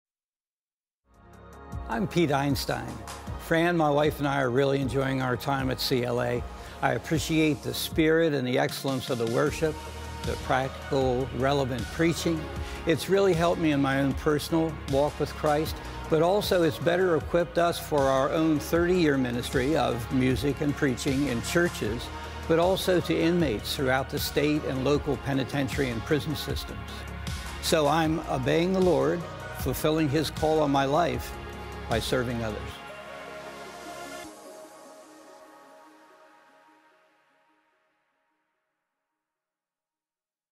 for the one Pete Einstein
Pete and his wife serve at church and also to prison inmates.  They know that they need to be connected to God's Word and His People in order to minister effectively.